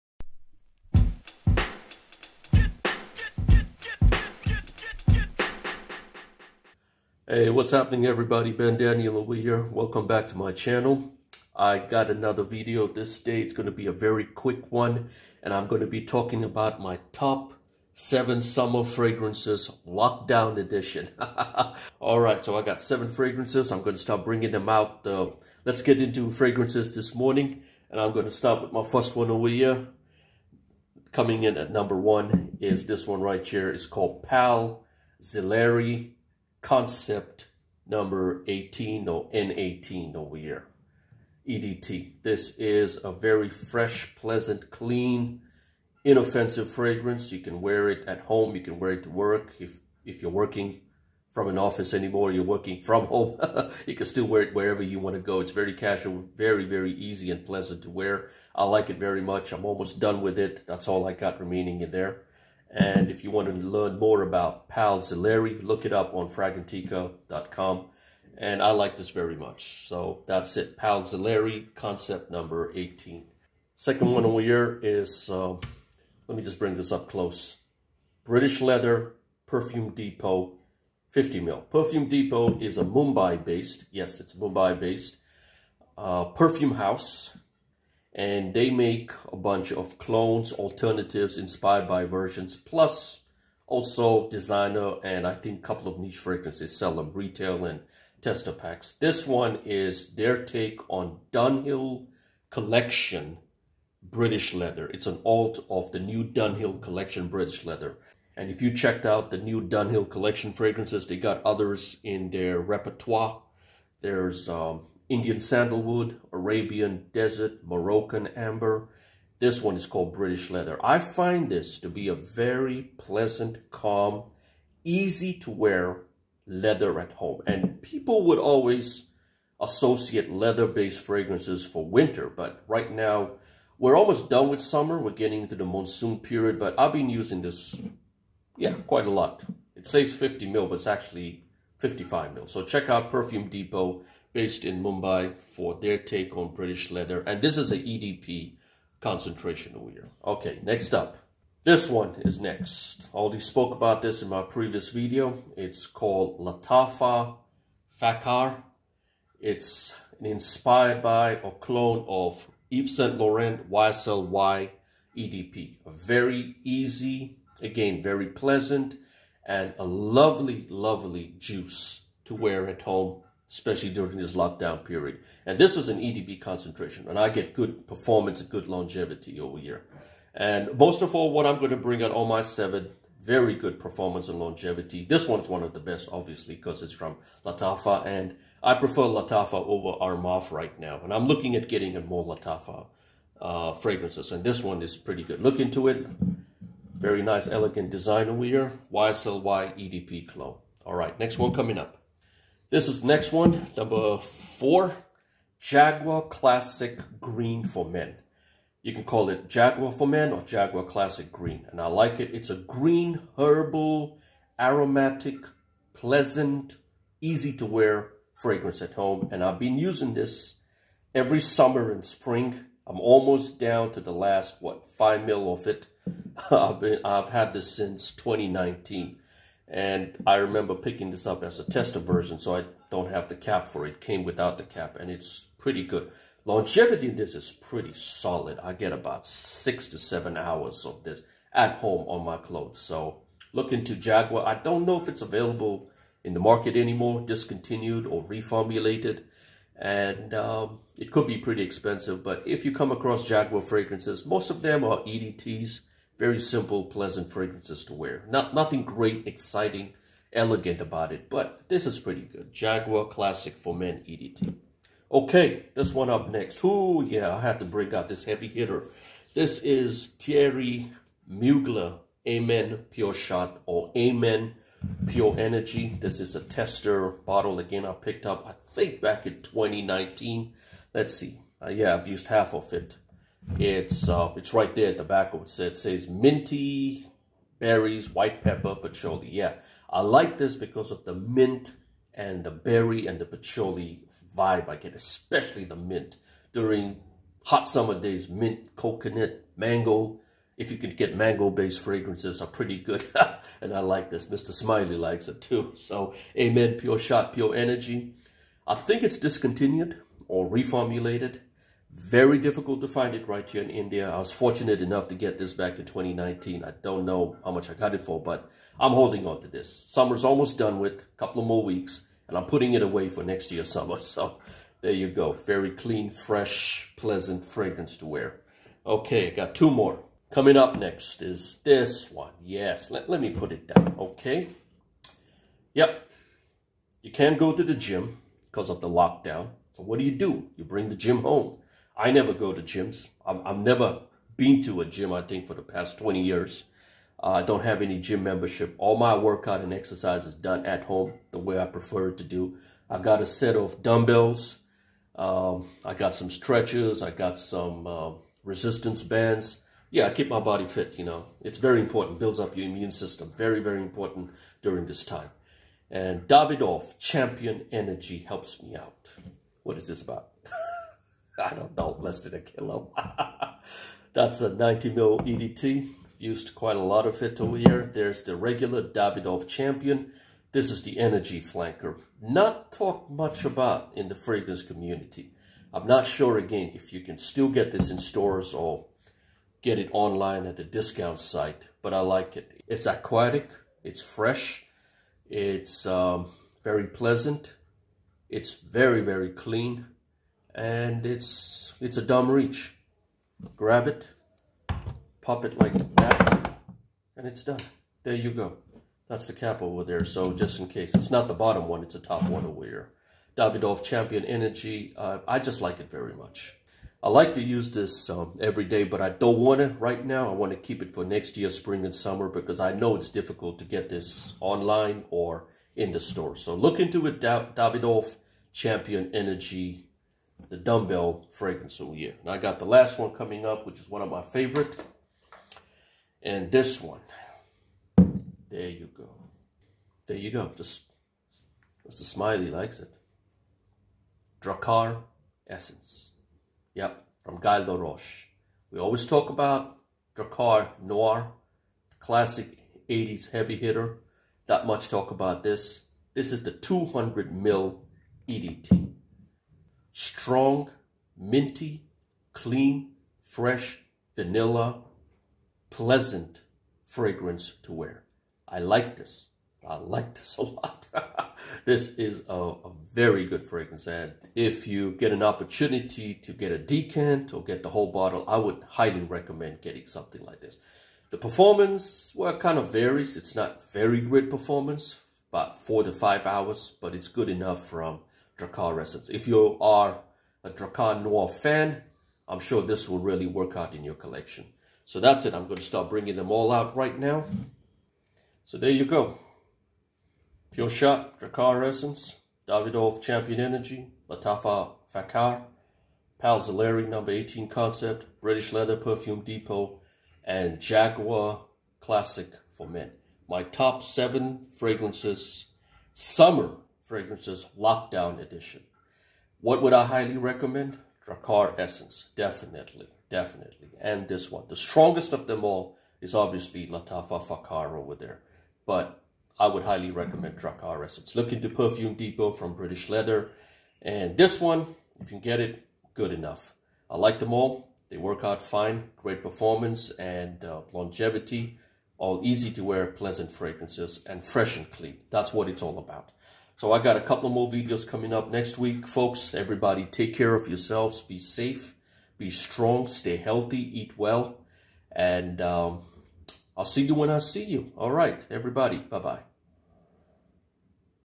My Top 7 Summer Fragrances - Lockdown Edition! | May 2021
Hey everyone, check out my most used summer frags during the lockdown time here in India. Stay safe and take care folks!